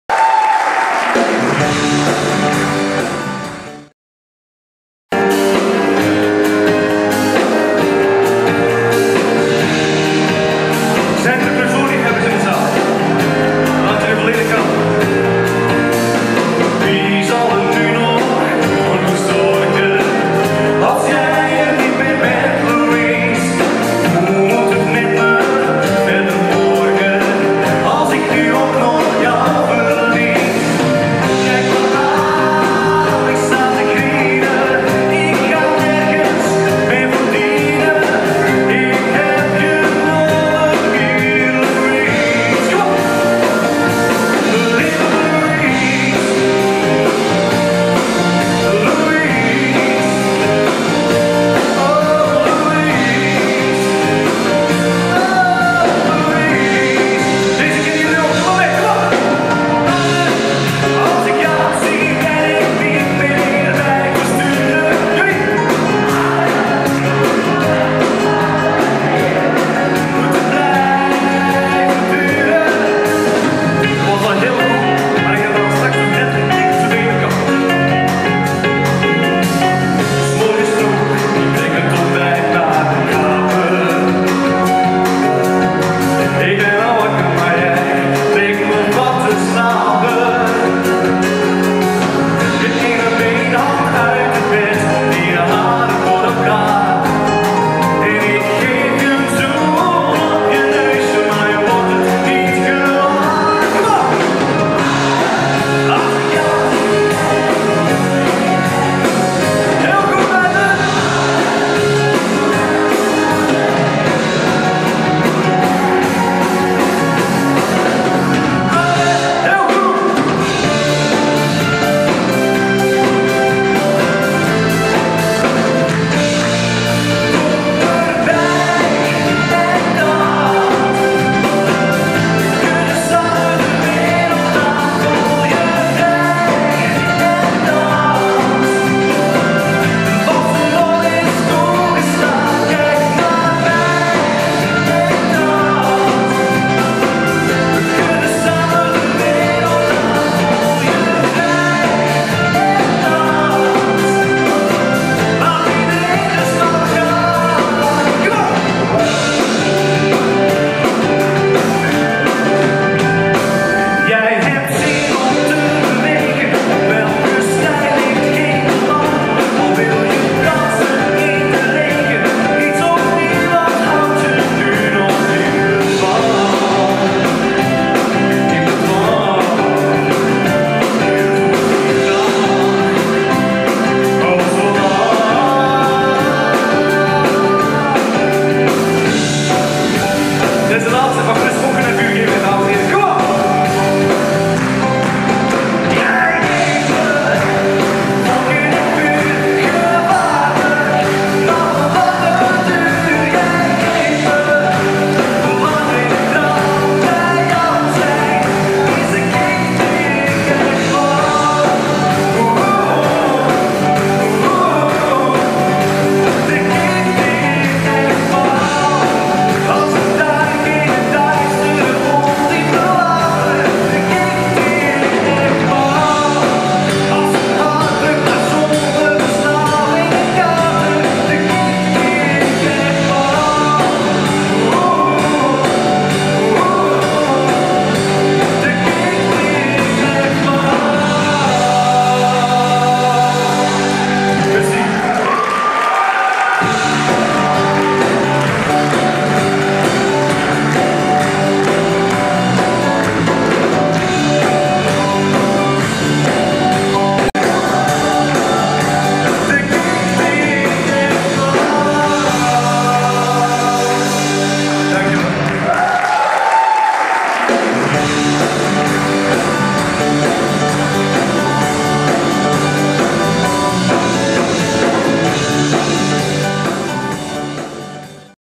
Alexander Metselaar Clouseau Medley
Caals dirigeert in het Kusttheater 't Colisée Blankenberge 2016